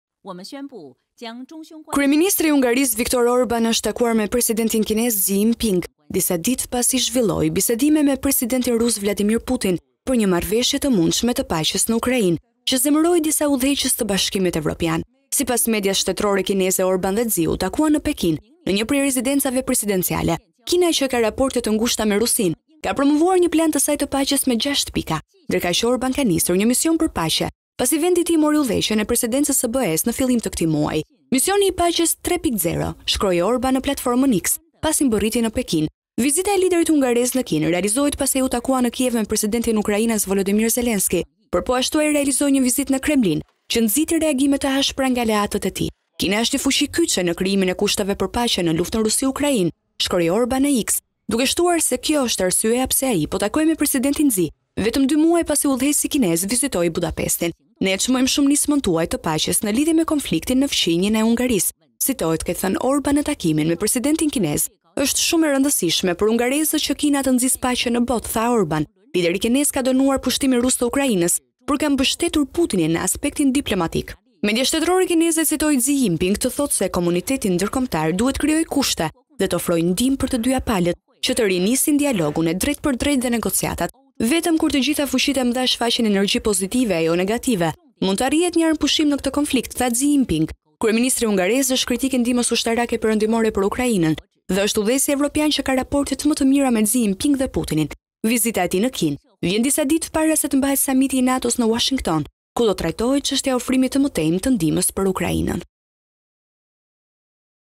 Top News/ Kërkon paqe për Ukrainën...në Kinë / Orban takon Xi Jinping pas Putinit e Zelenskit
Kryeministri i Hungarisë, Viktor Orban, është takuar me presidentin kinez, Xi Jinping, disa ditë pasi zhvilloi bisedime me presidentin rus, Vladimir Putin, për një marrëveshje të mundshme të paqes në Ukrainë, që zemëroi disa udhëheqës të Bashkimit Evropian. Sipas medias shtetërore kineze, Orban dhe Xi u takuan në Pekin në një prej rezidencave presidenciale. Kina, që ka raporte të ngushta me Rusinë, ka promovuar një plan të saj të paqes me gjashtë pika, ndërkaq Orban ka nisur një “mision për paqe”, pasi vendi i tij mori udhëheqjen e Presidencës së BE-së në fillim të këtij muaji. “Misioni i Paqes 3.0”, shkroi Orban në  platformën X  pasi mbërriti në Pekin. Vizita e liderit hungarez në Kinë realizohet pasi ai u takua në Kiev me presidentin ukrainas, Volodymyr Zelensky, por po ashtu ai realizoi një vizitë në Kremlin, që nxiti reagime të ashpra nga aleatët e tij. “Kina është një fuqi kyçe në krijimin e kushteve për paqe në luftën Rusi-Ukrainë”, shkroi Orban në X, duke shtuar se kjo është arsyeja pse ai po takohej me presidentin Xi vetëm dy muaj pasi udhëheqësi kinez vizitoi Budapestin. “Ne e çmojmë shumë nismën tuaj të paqes në lidhje me konfliktin në fqinjin e Hungarisë”, citohet të ketë thënë Orban në takimin me presidentin kinez. “Është shumë e rëndësishme për hungarezët që Kina të nxisë paqen në botë”, tha Orban. Lideri kinez ka dënuar pushtimin rus të Ukrainës, por ka mbështetur Putinin në aspektin diplomatik. Media shtetërore kineze, citoi Xi Jinping të thotë se “komuniteti ndërkombëtar duhet të krijojë kushte dhe të ofrojë ndihmë për të dyja palët që të rinisin dialogun e drejtpërdrejtë dhe negociatat”. “Vetëm kur të gjitha fuqitë e mëdha shfaqin energji pozitive e jo negative mund të arrihet një armëpushim në këtë konflikt”, tha Xi Jinping.
Kryeministri hungarez është kritik i ndihmës ushtarake perëndimore për Ukrainën dhe është udhëheqësi evropian që ka raportet më të mira me Xi Jinping dhe Putinin. Vizita e tij në Kinë vjen disa ditë para se të mbahet samiti i NATO-s në Uashington, ku do të trajtohet çështja e ofrimit të mëtejmë të ndihmës për Ukrainën.

Pamje: AFP